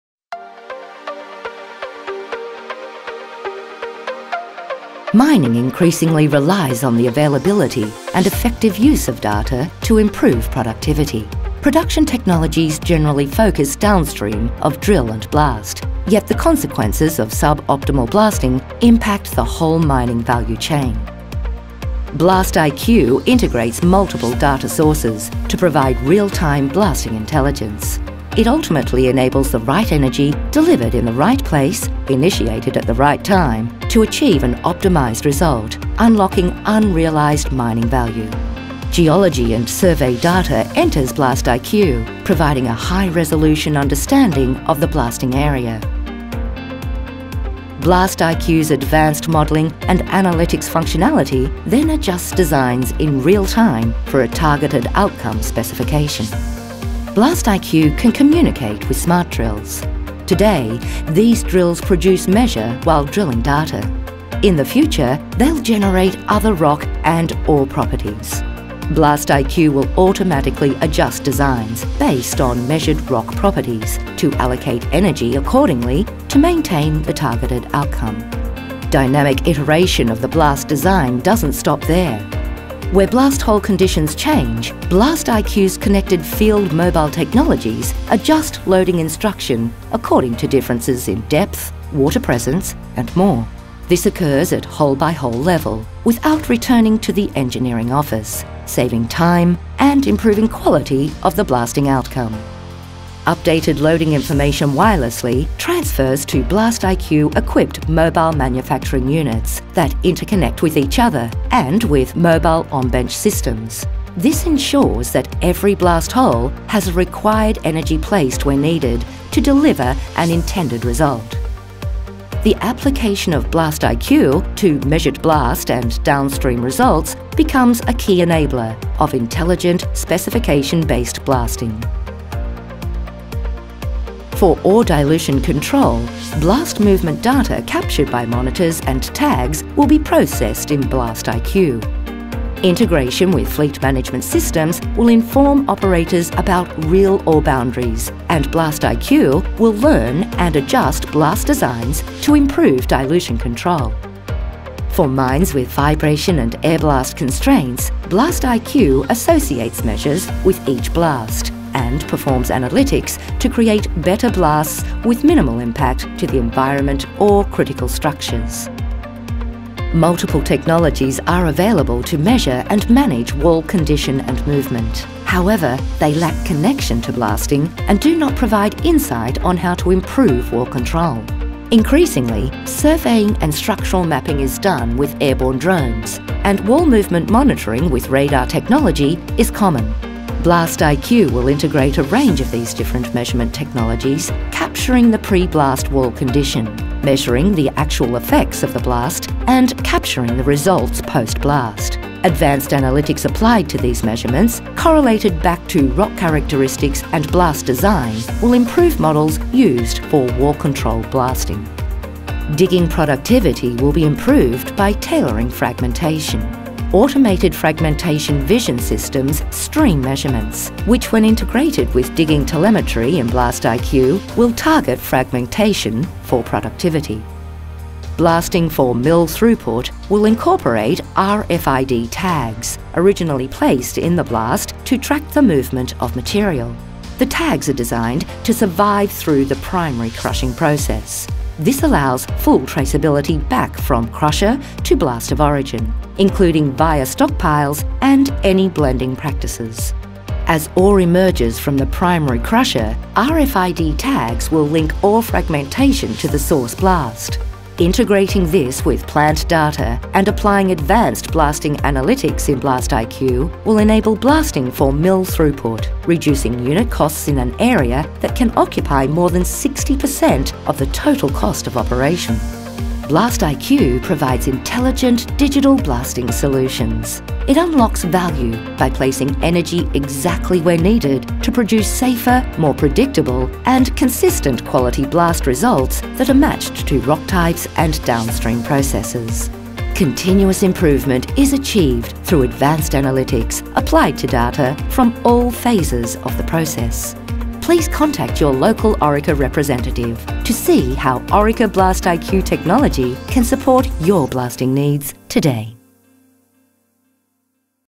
Our vision for digitally enabled better blasting – powered by BlastIQ™
Our vision is to partner with our customers to dynamically optimise the drill and blast process. BlastIQ™ is designed to enable continuous improvement of blasting outcomes by integrating data and insights from digitally connected technologies across the drill and blast process.

Solutions powered by the BlastIQ™ platform can deliver sustainable improvements that reduce the overall cost of the drill and blast operations, improve productivity and facilitate regulatory compliance.

To find out more about BlastIQ™, please contact your local Orica representative. to learn more.